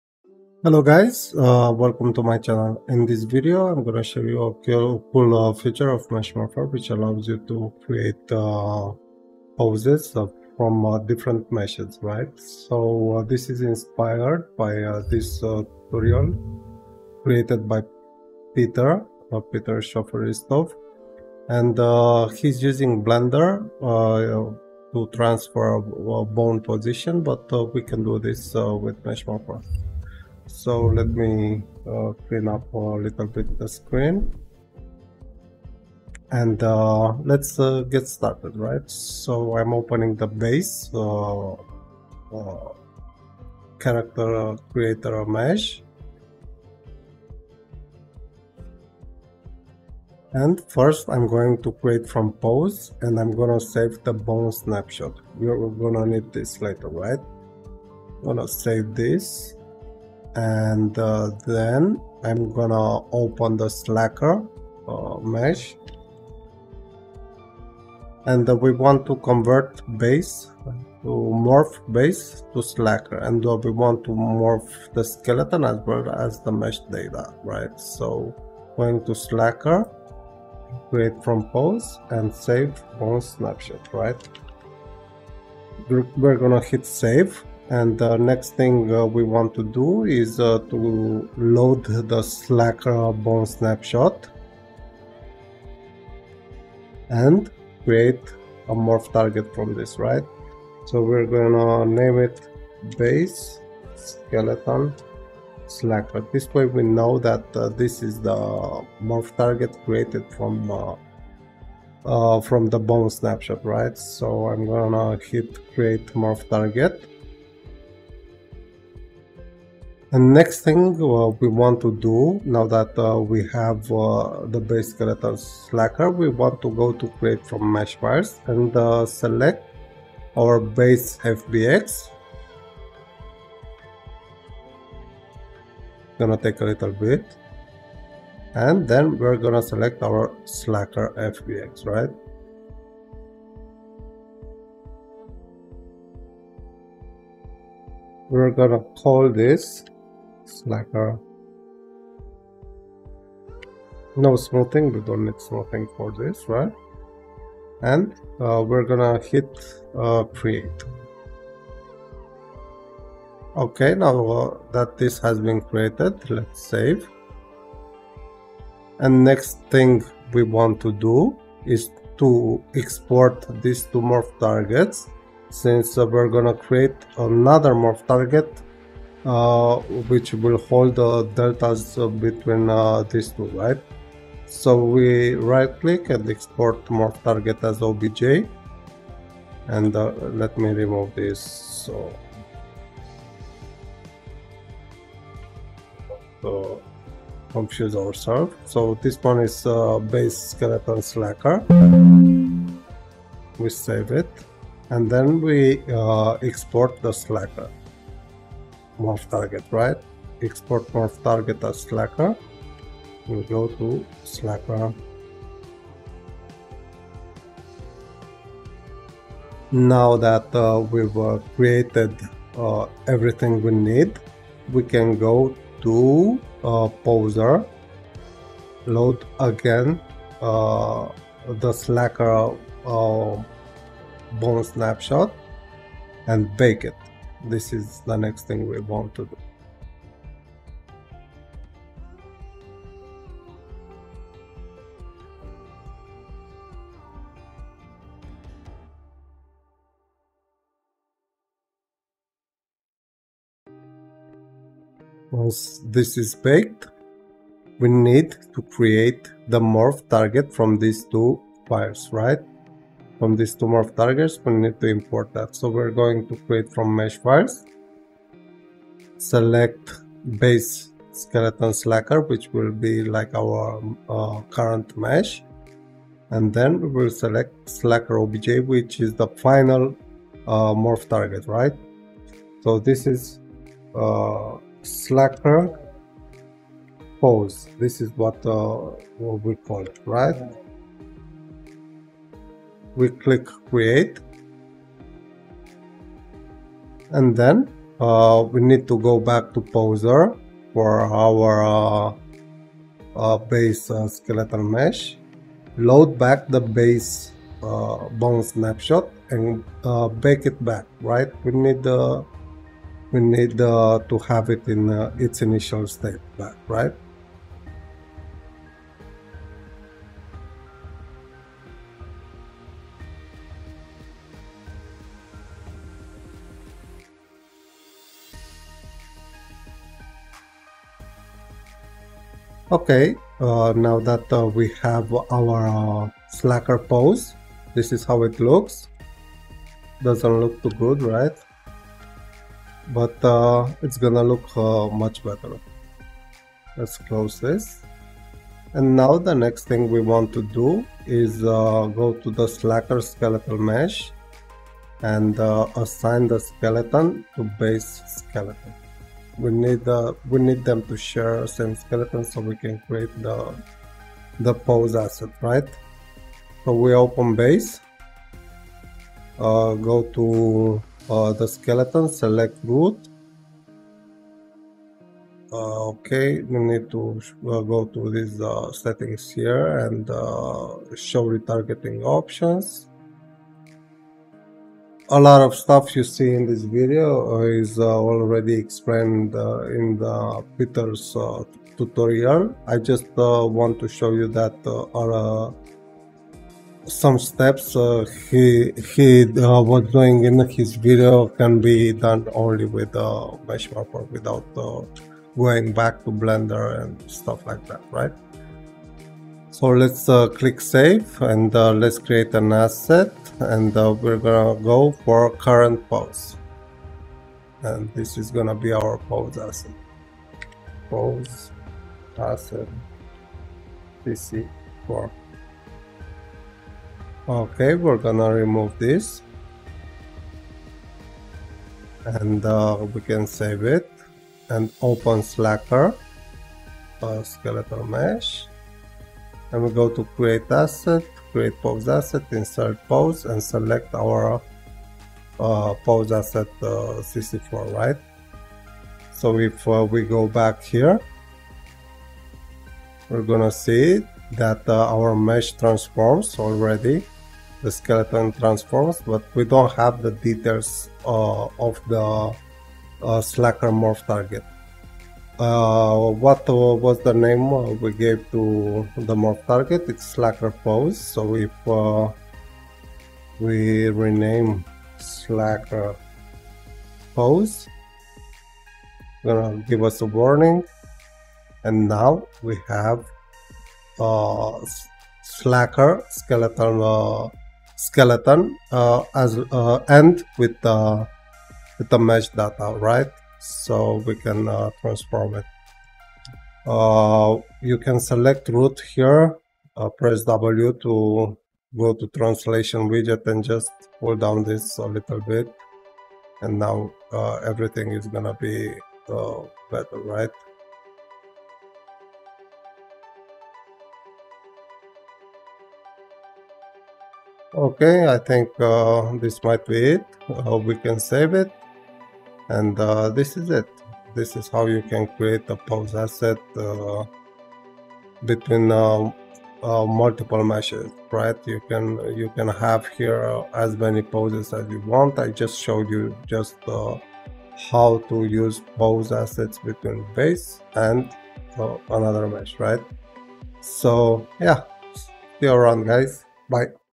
(Tutorial) Mesh Morpher - Create Poses from Meshes
This video tutorial shows how we can create Poses from Meshes using Mesh Morpher